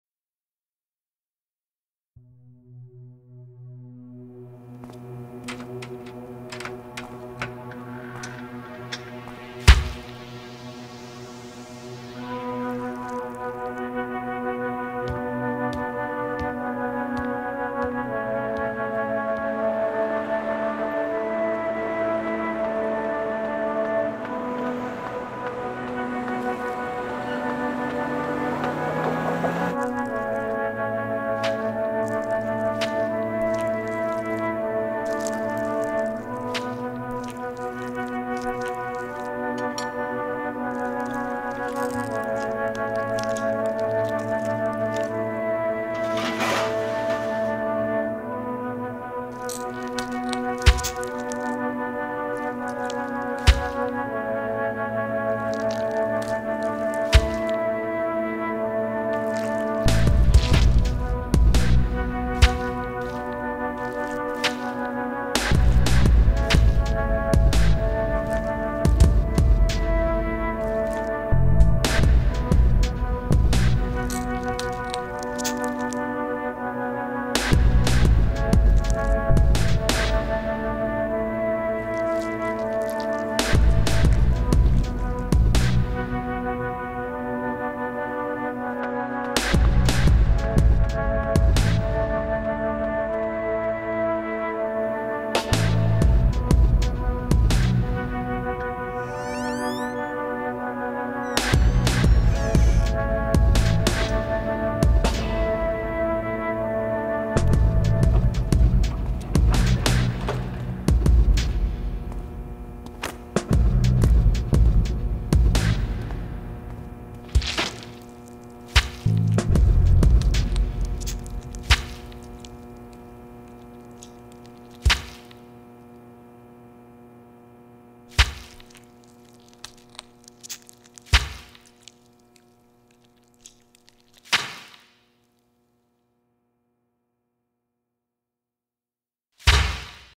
DELI X Beta Unit present The Butcher Shop
The Butcher Shop

Staying within the delicatessen idea, DELI collaborated with California based brand BETA Unit to produce "The Butcher Shop!" The Butcher Shop is a contemporary take on a back kitchen/food prep area; that will only carry exclusive styles from the two brands. Taking it a step further, we have created a short film, which the store space is themed from.

The Butcher Shop is scheduled to close doors May 30th, 2010

Sound Design & Soundtrack by:

Benjamin Woodward